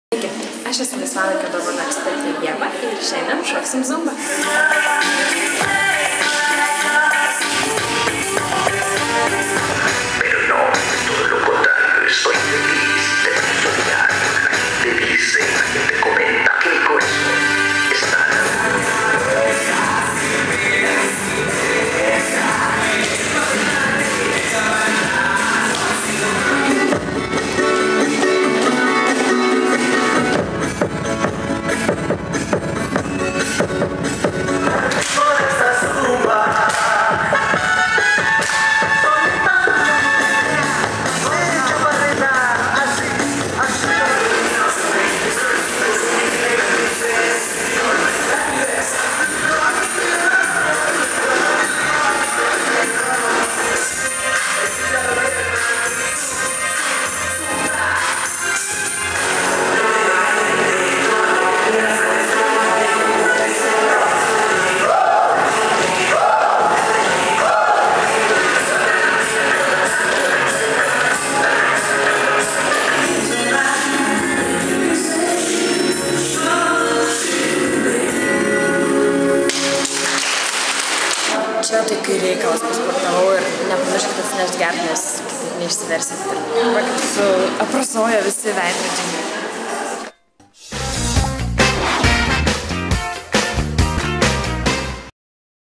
ZUMBA Fitness® šokių pamoka
ZUMBA Fitness® - tai didžiausia šventė Jūsų kūnui ir palaima sielai!
Energinga, užvedanti lotynų Amerikos šokių muzika, profesonalūs treneriai, nesudėtingi, lotynų Amerikos šokių žingsneliai, smagūs, ritmiški judesiai, skirti visoms raumenų grupėms sustiprinti ir begalė gerų, teigiamų emocijų. Unikali programa kviečia mėgautis kiekviena šokio teikiama akimirka tuo pačiu metu intensyviai sportuojant. ZUMBA Fitness® treniruotėse Jūs ne tik šoksite, sportuosite, bet ir pasijusite kaip tikrame vakarėlyje!
Skirkite laiko sau, o Jūsų kūnas ir siela Jums padėkos!